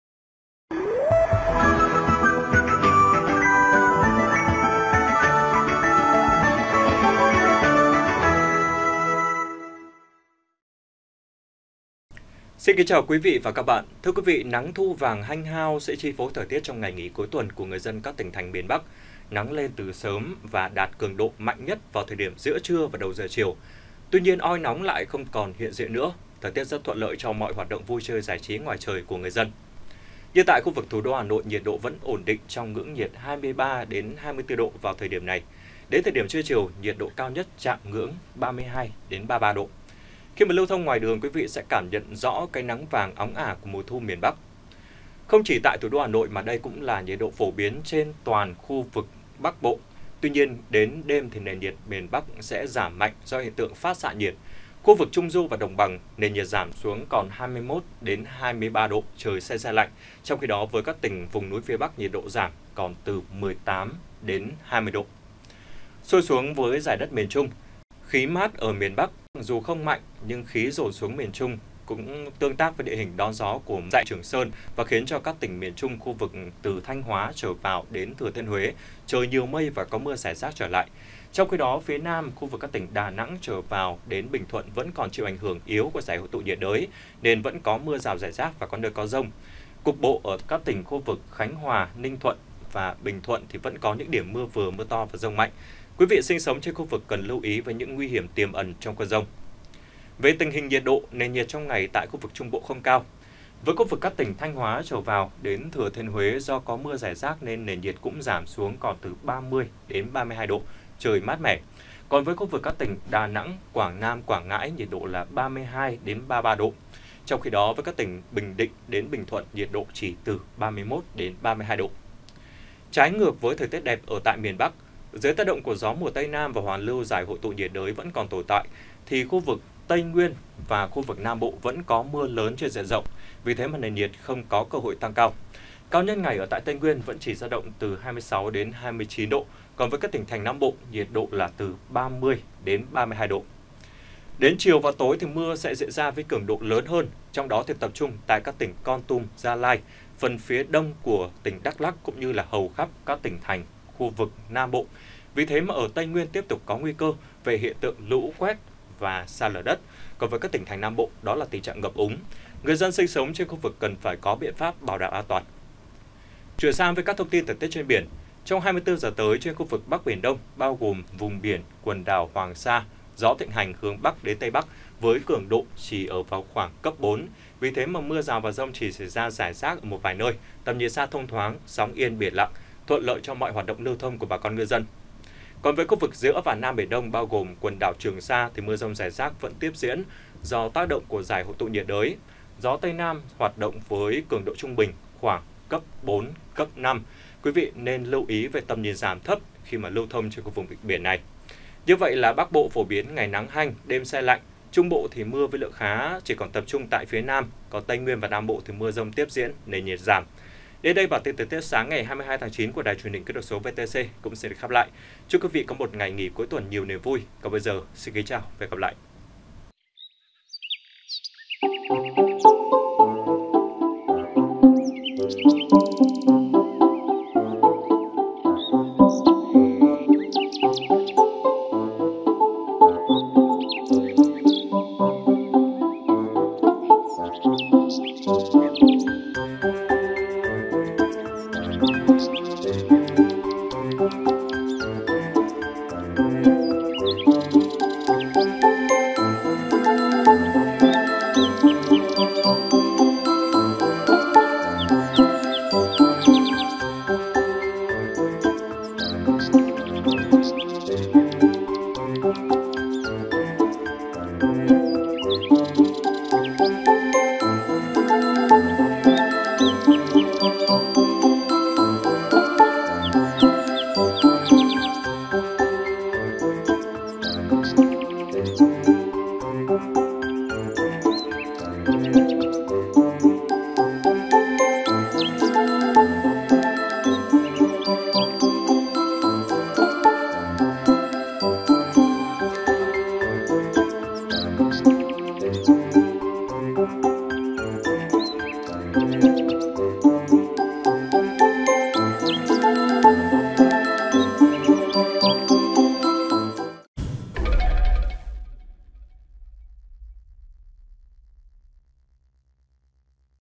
Thời tiết 6h 22/09/2019: Nhiệt độ Hà Nội thấp nhất 23 độ, se lạnh vào sáng sớm| VTC14
VTC14 |Nắng Thu vàng hanh hao sẽ chi phối thời tiết trong ngày nghỉ cuối tuần của người dân các tỉnh thành miền Bắc. Nắng lên từ sớm và đạt cường độ mạnh nhất vào thời điểm giữa trưa và đầu giờ chiều. Tuy nhiên oi nóng lại không còn hiện diện nữa. Thời tiết rất thuận lợi cho mọi hoạt động vui chơi giải trí ngoài trời của người dân.
Như tại thủ đô Hà Nội, nhiệt độ vẫn ổn định trong ngưỡng 23 – 24 độ vào thời điểm này. Đến trưa chiều, nhiệt cao nhất chạm ngưỡng 32 – 33 độ. Khi lưu thông ngoài đường quý vị sẽ cảm nhận rõ cái nắng vàng óng ả của mùa Thu miền Bắc.